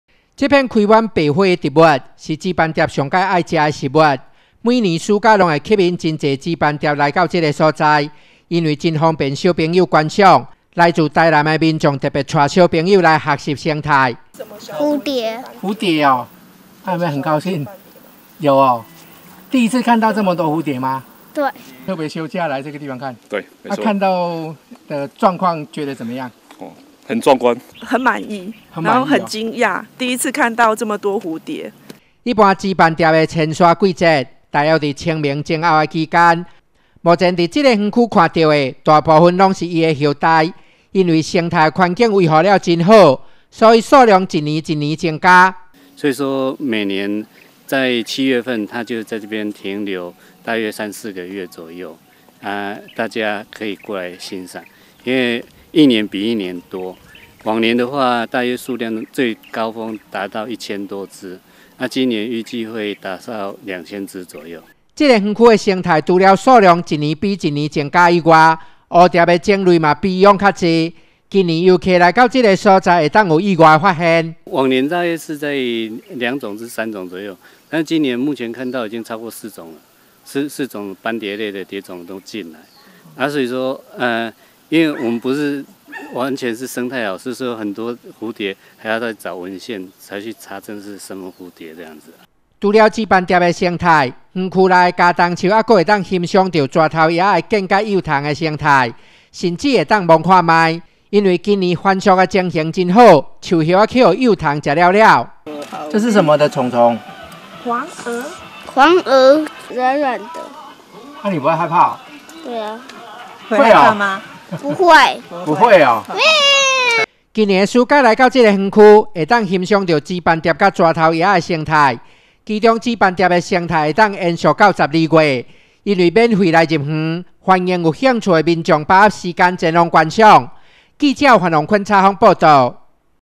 雲林新聞網-古坑華山紫斑蝶季
每年紫斑蝶的遷移，從高雄茂林出發，沿路會經過雲林古坑、林內等，為了要營造古坑華山地區紫斑蝶生態，古坑華山休閒產業促進會理事長李契螢，三年前，開始在厝後綠地陸續栽種上千棵的台灣澤蘭，紫斑蝶的數量，從二百、五百、一千的增加。今年暑假花開的季節，數量可望繼續增加，紫蝶翩翩的情形，相當的壯觀，吸引遊客前來觀賞